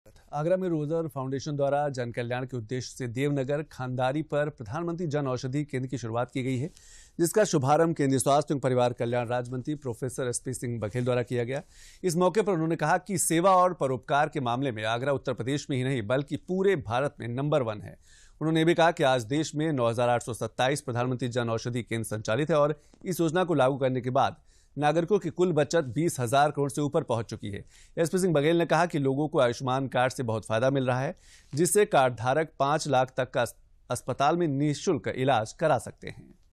AGRA में प्रधानमंत्री जन औषधि केंद्र की हुई शुरूआत
आगरा में रोजर फाउंडेशन द्वारा जनकल्याण के उद्देश्य से देव नगर, खंदारी पर प्रधानमंत्री जन औषधि केंद्र की शुरुआत की गई, जिसका शुभारम्भ केंद्रीय स्वास्थ्य एवं परिवार कल्याण राज्यमंत्री प्रो. एसपी सिंह बघेल द्वारा किया गया। इस मौके पर उन्होंने कहा कि सेवा और परोपकार के मामले में आगरा उत्तर प्रदेश  में ही नहीं बल्कि पूरे भारत में नंबर बन है। उन्होंने कहा कि आज देश में 9827 प्रधानमंत्री जन औषधि केंद्र संचालित  हैं और  इस योजना को लागू करने के बाद नागरिकों की कुल बचत 20 हजार करोड़ से ऊपर पहुँच  चुकी है।   एसपी सिंह बघेल ने कहा कि लोगों को आयुष्मान कार्ड से बहुत फायदा मिल रहा है जिससे कार्डधारक  पांच लाख तक का अस्पताल में निःशुल्क  इलाज  करा सकता है।